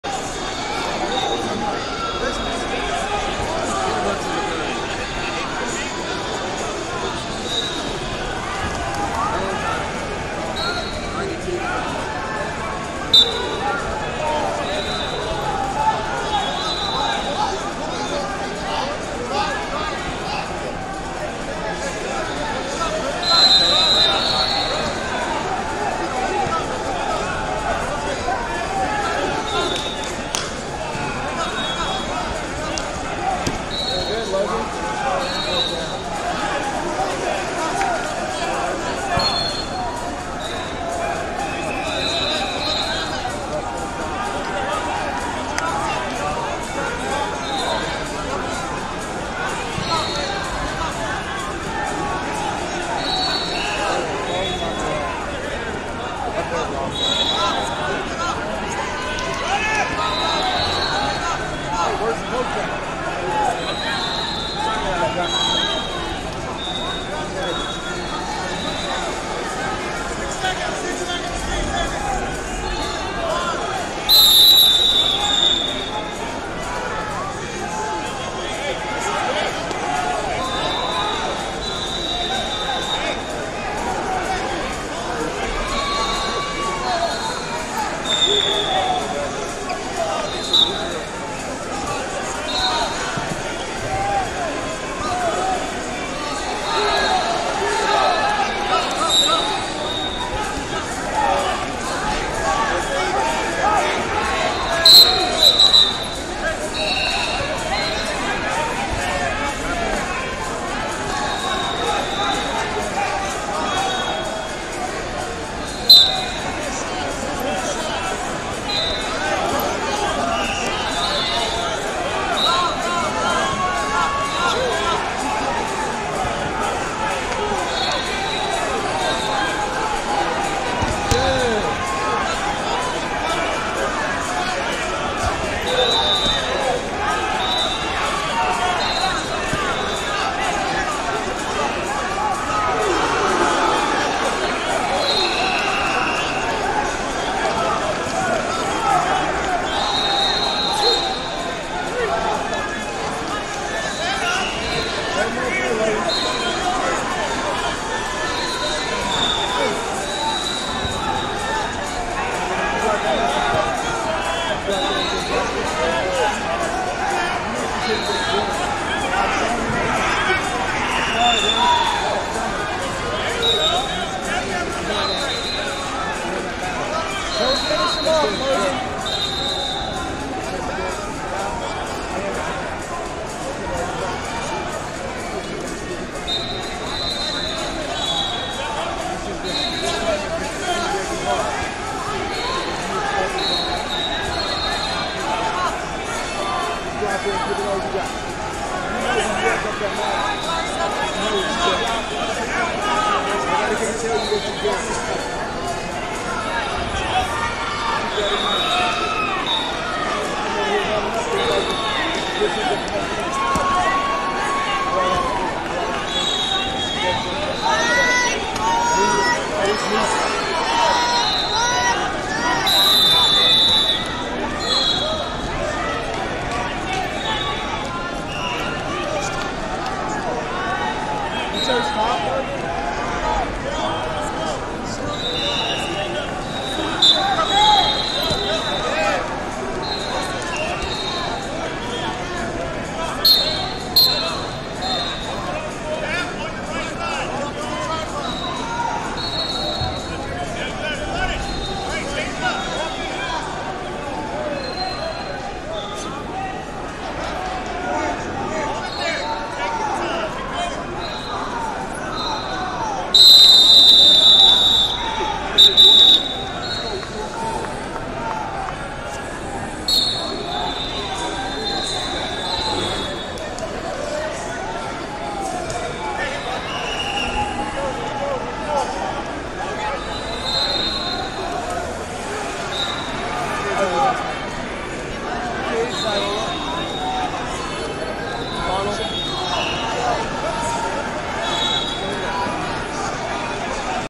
152 Lbs Cons. Round 3 - Jack M. Ringger, TX Vs Logan Soileau, TX C7bb
Flosports-partner-access/2022 Brian Keck Memorial Preseason Nationals-Recruiting Showcase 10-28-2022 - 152 Lbs Cons. Round 3 - Jack M. Ringger, TX Vs Logan Soileau, TX C7bb